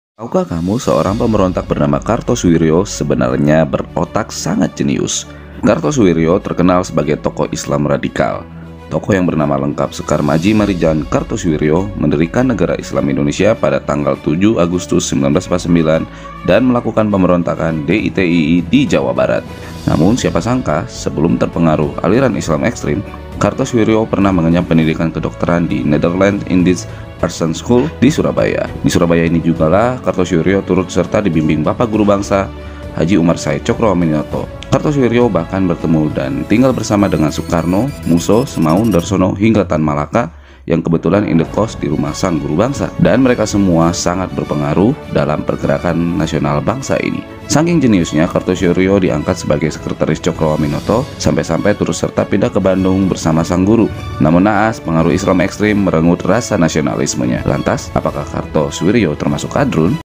KARTOSUWIRYO TERNYATA SANGAT JENIUS
video ini menjelaskan tentang sisi lain kartosuwiryo di luar pemberontakan DI/TII